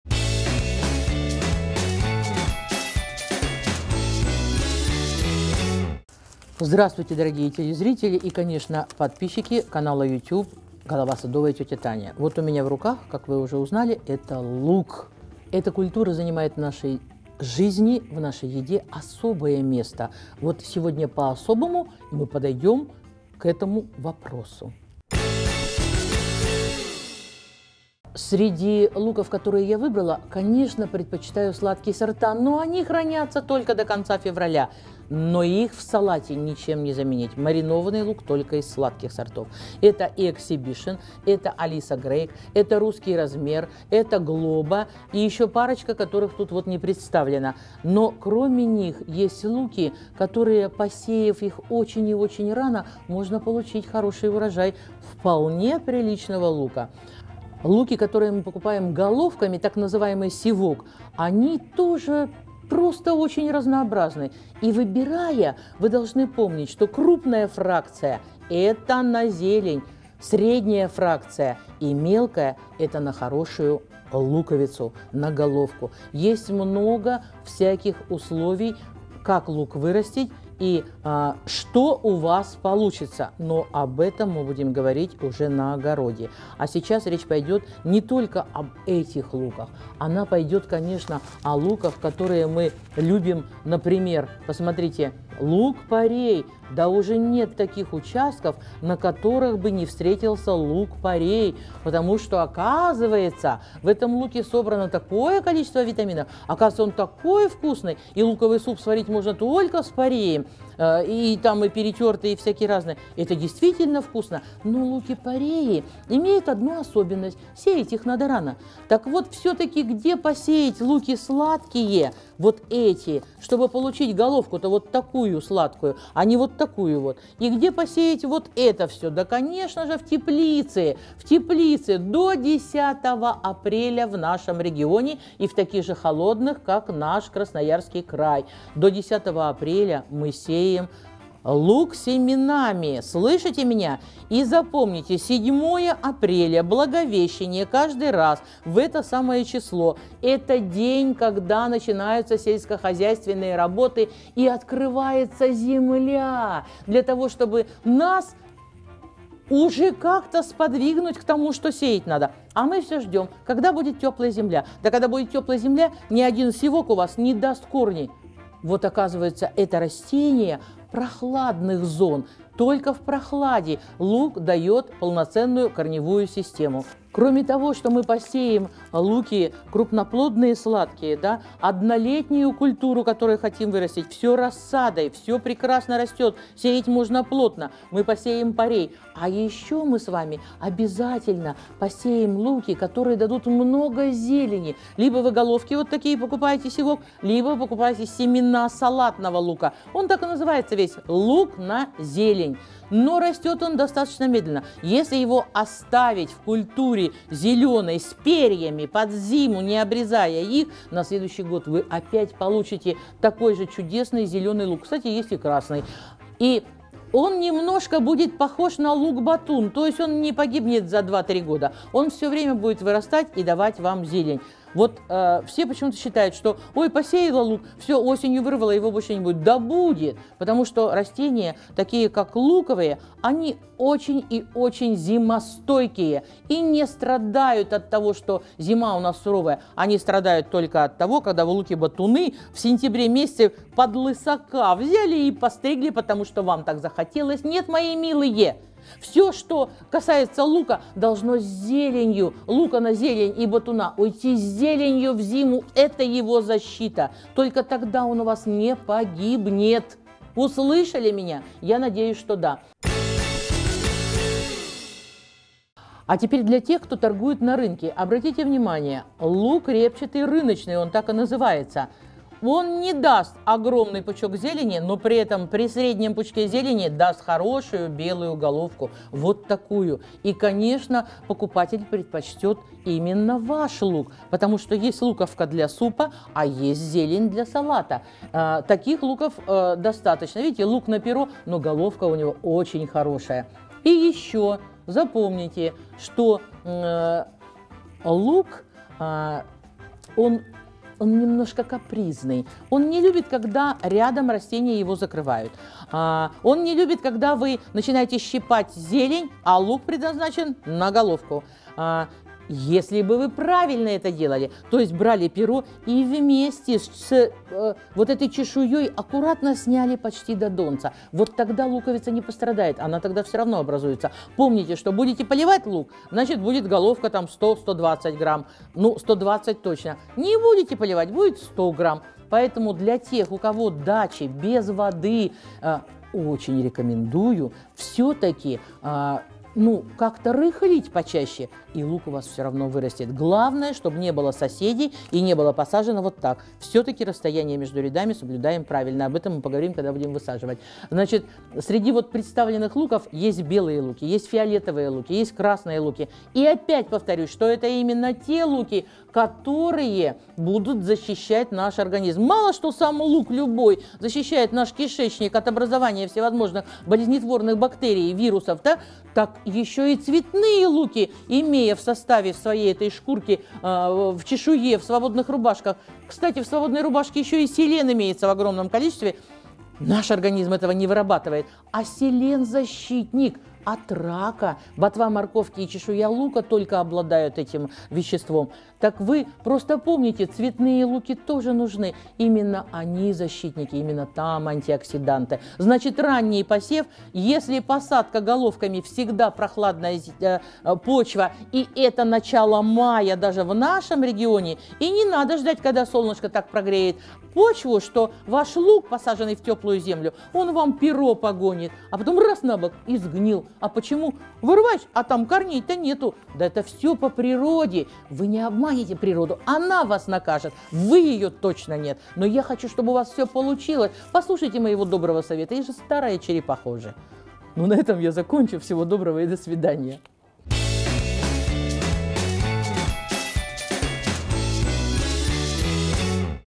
Голова садовая - Как правильно выбрать семена лука
Сегодня я расскажу вам, о семенах лука, и о луке в целом. Как выбрать семена, как и когда заниматься посадкой. Как ухаживать за луком.